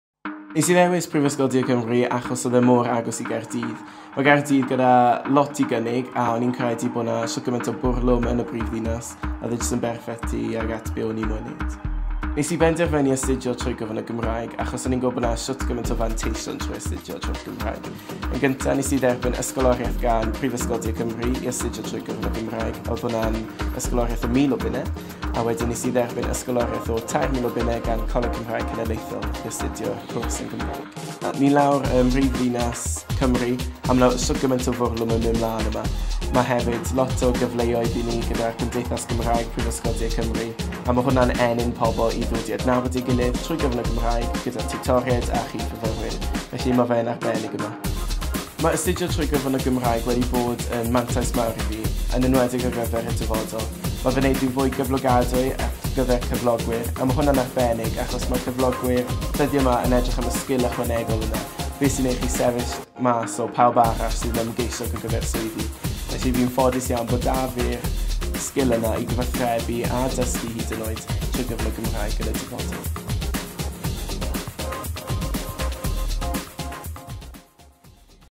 Cyfleoedd Cymraeg ym Mhrifysgol De Cymru
Mae nifer o gyfleoedd i fyfyrwyr astudio trwy gyfrwng y Gymraeg ym Mhrifysgol De Cymru. Dyma flas ar yr opsiynau sydd ar gael - gallwch gael mwy o wybodaeth mewn Diwrnod Agored

There are lots of opportunities for students to study through the medium of Welsh at the University of South Wales. Here’s a taste of what’s available – you can get more information at an Open Day